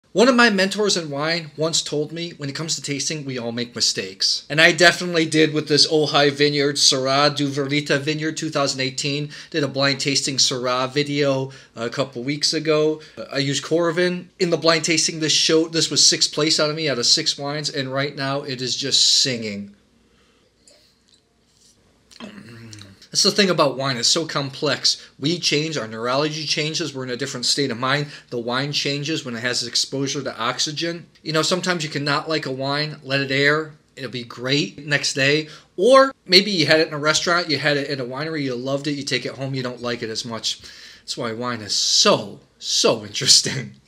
I Made a Mistake With This Wine
Wines featured in this episode:
The Ojai Vineyard, Duvarita Vineyard Syrah 2018